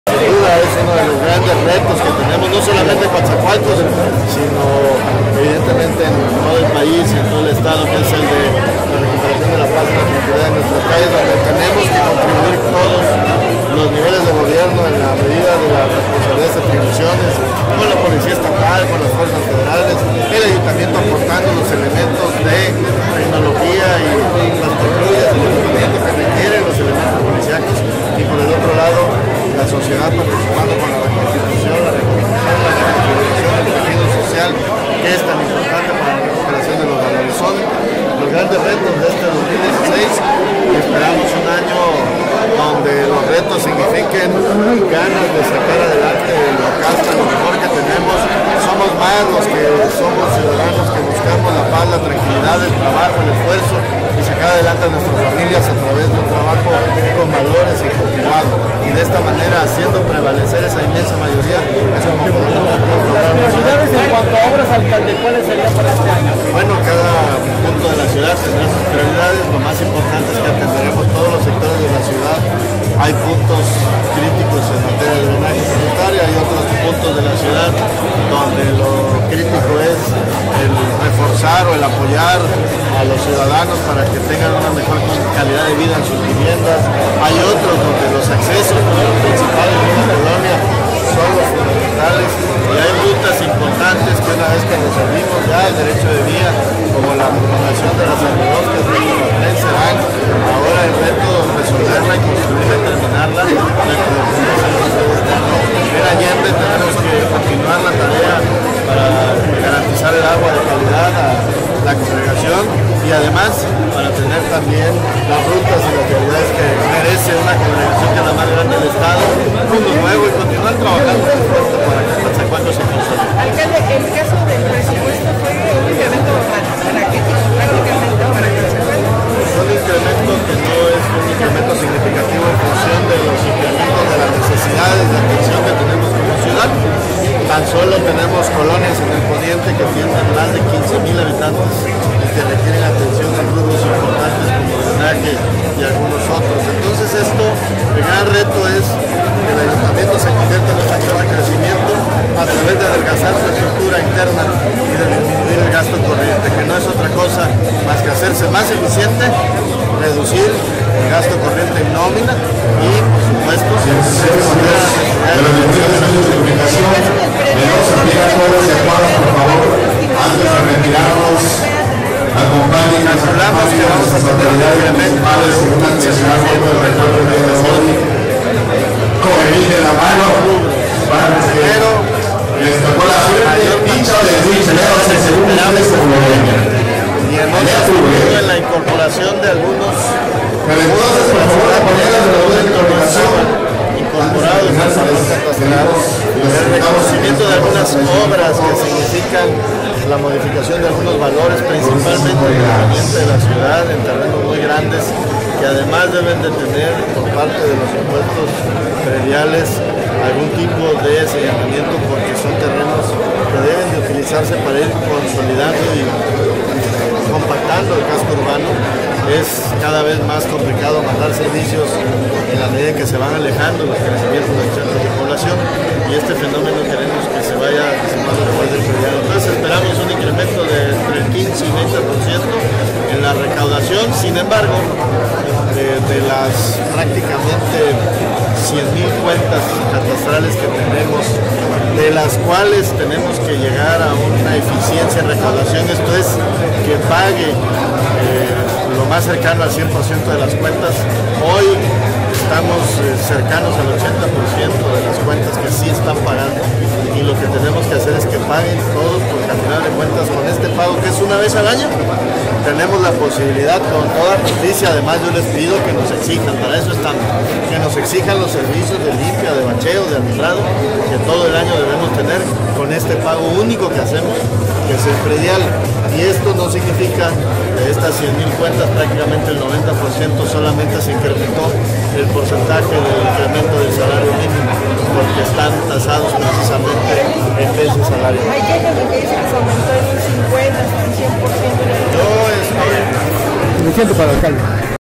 Que Paguen el Predial y que También Exijan Obras: Joaquin Caballero
Coatzacoalcos, Ver.- El alcalde de Coatzacoalcos Joaquín Caballero Rosiñol, en entrevista realizada después de cortar la rosca de Reyes con reporteros de la fuente, hablo del manejo de los recursos que su administración ejercerá durante este 2016,  de las acciones que en el tema de seguridad realizarán los tres ordenes de gobierno para rescatar al municipio de la violencia e inseguridad que afecta a los ciudadanos.

De igual forma el municipe porteño se refirió al programa de obras que llevara a cabo su gobierno, destacando el tema de la reparación de las vialidades en las colonias, así como el de los problemas de drenaje sanitario sin olvidar el mejorar la calidad de vida de los ciudadanos.

Caballero Rosiñol, hizo especial mención en el tema del incremento en el pago del impuesto predial,

“Fue un incremento que no es un incremento significativo, en función del incremento de las necesidades que tenemos como ciudad”,

Refifrió que esperan un incremento entre el 15 o 20 % en la recaudacion, eficienti zando los servicio para cobro, sin embargo de las 100 mil cuentas catastrales, solo un 80 por ciento es el que paga en tiempo.

" Y lo que tenemos que hacer es que paguen todos,  porque al final de cuentas con éste pago que es una vez al año, tenemos la posibilidad con toda justicia y además yo les pidos que nos exijan, para eso estamos, que nos exijan los servicios de limpia, de bacheo,  de alumbrado, que todo el año debemos tener,  con este pago único  que hacemos que es el predial", expuso.